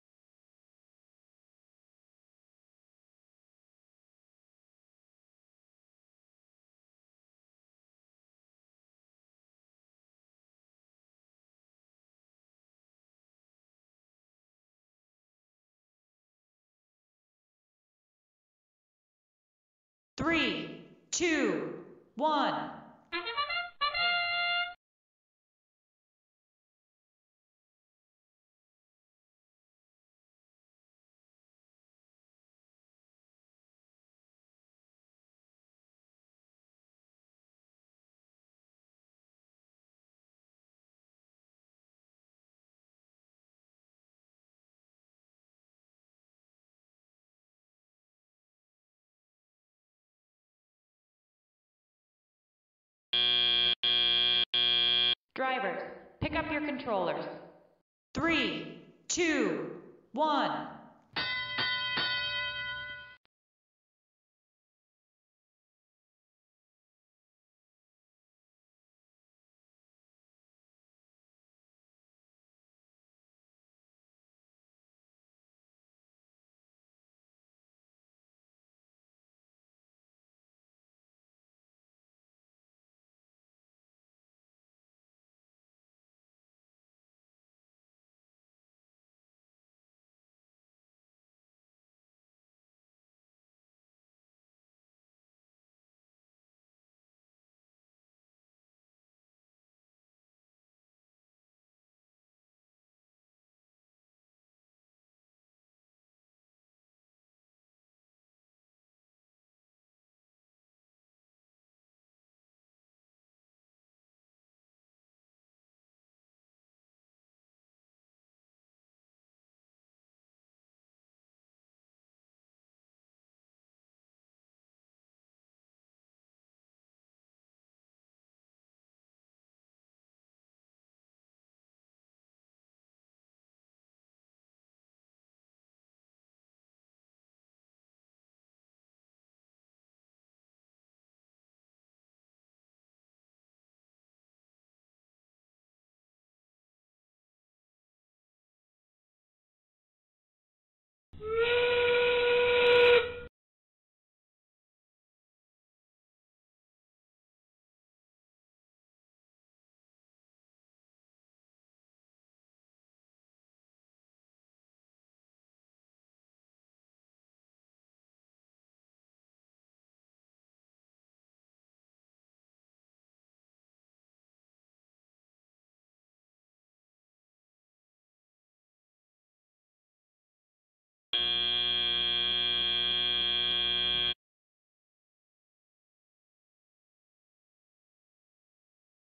Jeff & Nancy Show | Match 5
Match 5 for the Jeff & Nancy Show Qualifier
Score: 160

0:00​​ Setup
0:20​​ Autonomous
0:58​​ Driver Control
3:06​​ Scores